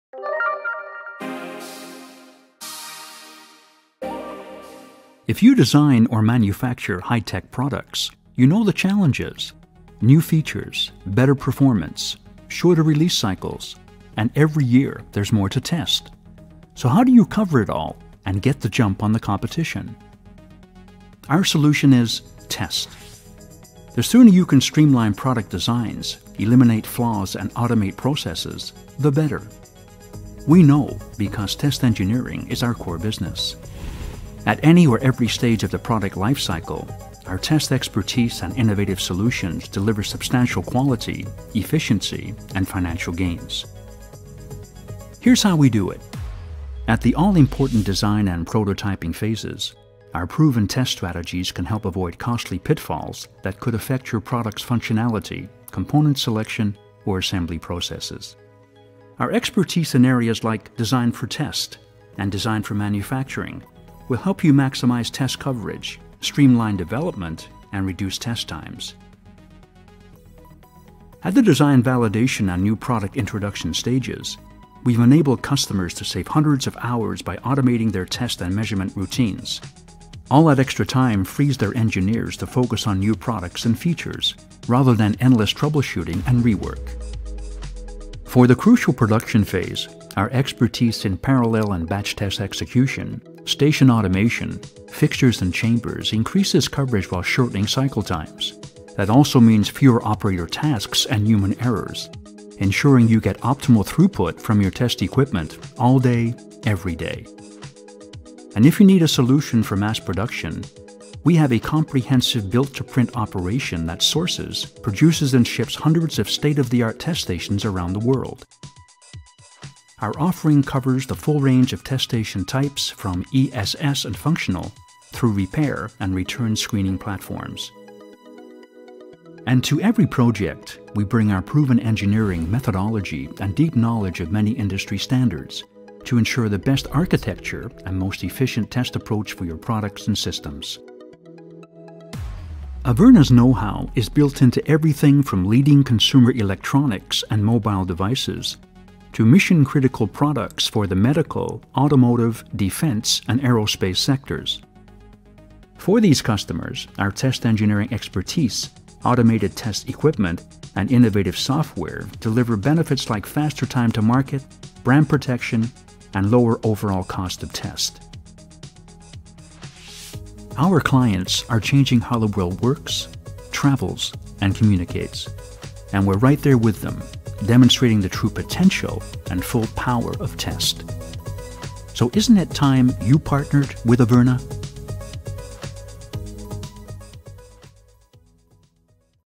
Test Engineering Services for the Entire Product Lifecycle
Our clients are changing how the world works, travels and communicates. And we're right there with them -- demonstrating the true potential and full power of Test. Watch this video to see how we do it.

If you design or manufacture high-tech products, you know the challenges -- new features, better performance, shorter release cycles.

And every year, there's more to test. So how do you cover it all and get the jump on the competition?

Our solution is Test. The sooner you can streamline product designs, eliminate flaws and automate processes, the better.

We know because Test Engineering is our core business.

At any or every stage of the product lifecycle, our test expertise and innovative solutions deliver substantial quality, efficiency and financial gains.

Here's how we do it.

At the all-important design and prototyping phases, our proven test strategies can help avoid costly pitfalls that could affect your product's functionality, component selection, or assembly processes.

Our expertise in areas like Design for Test and Design for Manufacturing will help you maximize test coverage, streamline development and reduce test times.

At the design-validation and new-product-introduction (NPI) stages, we've enabled customers to save hundreds of hours by automating their test and measurement routines.

All that extra time frees their engineers to focus on new products and features, rather than endless troubleshooting and rework.

For the crucial production phase, our expertise in parallel and batch test execution, station automation, fixtures and chambers increases coverage while shortening cycle times.

That also means fewer operator tasks and human errors, ensuring you get optimal throughput from your test equipment, all day, every day.

And if you need a solution for mass production, we have a comprehensive build-to-print operation that sources, produces and ships hundreds of state-of-the-art test stations around the world.

Our offering covers the full range of test station types, from ESS and functional, through repair and returns-screening platforms.

And to every project we bring our proven engineering methodology and deep knowledge of many industry standards to ensure the best architecture and most efficient test approach for your products and systems.

Averna's know-how is built into everything from leading consumer electronics and mobile devices to mission-critical products for the medical, automotive, defense and aerospace sectors.

For these customers, our test engineering expertise, automated test equipment, and innovative software deliver benefits like faster time to market, brand protection and lower overall cost of test.
Our clients are changing how the world works, travels, and communicates.

And we're right there with them -- demonstrating the true potential and full power of Test.

So isn't it time you partnered with Averna?

To learn more about Averna's Test Engineering services and solutions,

English and French Subtitles Available.